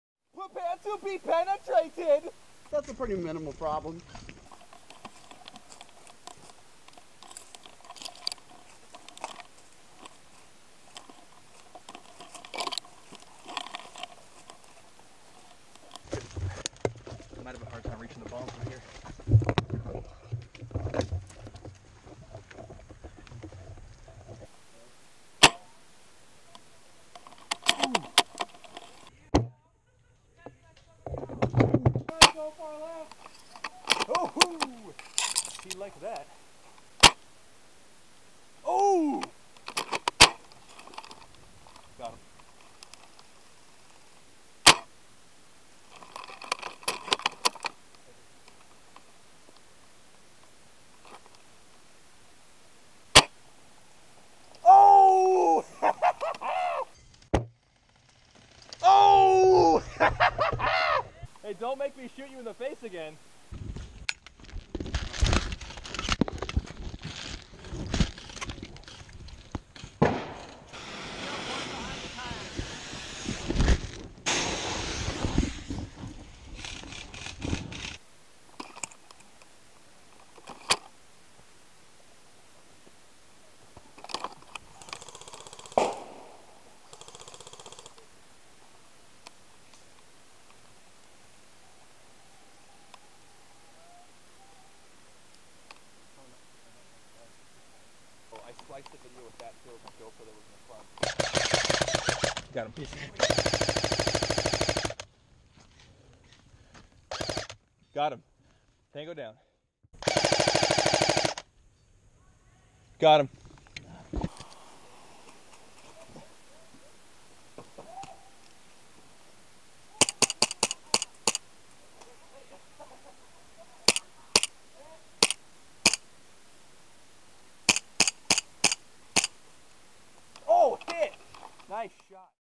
The Mill 2/22/17
Lots of fun at The Mill!

Action with my VSR10, Grease Gun, grenades and 1911!